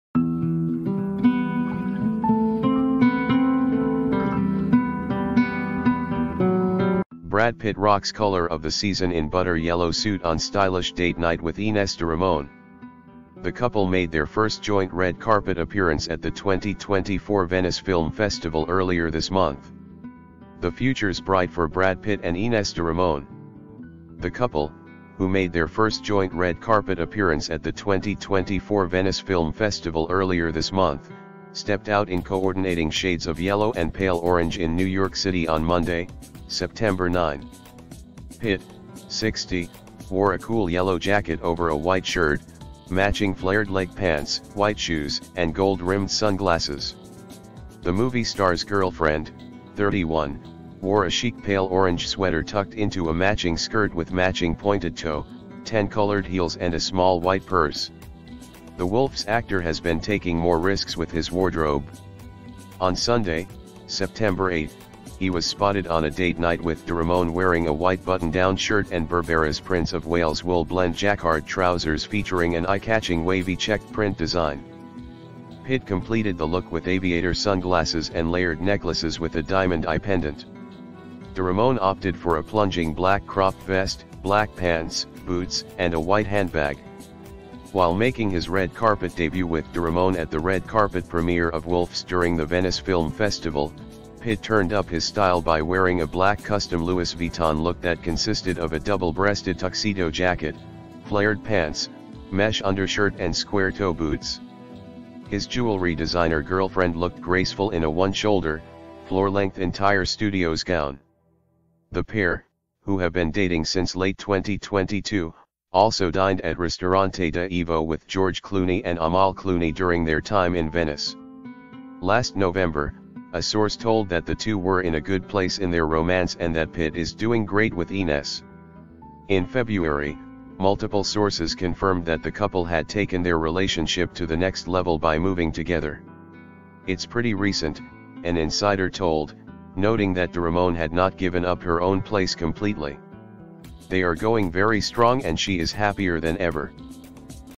🌟Brad Pitt Rocks Color of the Season in Butter Yellow Suit on Stylish Date Night with Ines de Ramon🌟
Brad Pitt and Ines de Ramon are turning heads with their chic and coordinated fashion choices. The couple made their debut on the red carpet together at the 2024 Venice Film Festival and have continued to showcase their stylish appearances since then.

On September 9, the pair was seen stepping out in New York City, where Pitt dazzled in a butter yellow jacket paired with flared-leg pants, a white shirt, and gold-rimmed sunglasses. De Ramon complemented him with a pale orange sweater and skirt, tan-colored heels, and a white purse.

Earlier, on September 8, Pitt was spotted on another date night, sporting a white button-down shirt and distinctive Burberry trousers featuring a wavy checked print. He accessorized with aviator sunglasses and layered necklaces, including a diamond "I" pendant. Meanwhile, de Ramon opted for a plunging black cropped vest, black pants, and boots.

Their red carpet debut at the Venice Film Festival was marked by Pitt’s bold fashion choice of a custom Louis Vuitton black tuxedo, complete with flared pants and a mesh undershirt. De Ramon looked elegant in a one-shoulder, floor-length gown from Entire Studios. During their Venice visit, they also dined with George and Amal Clooney at Ristorante Da Ivo.

The couple, who began dating in late 2022, are reportedly thriving in their relationship. Sources have indicated that they have recently moved in together, though de Ramon still maintains her own residence. The romance appears to be strong, with both parties seemingly happier than ever.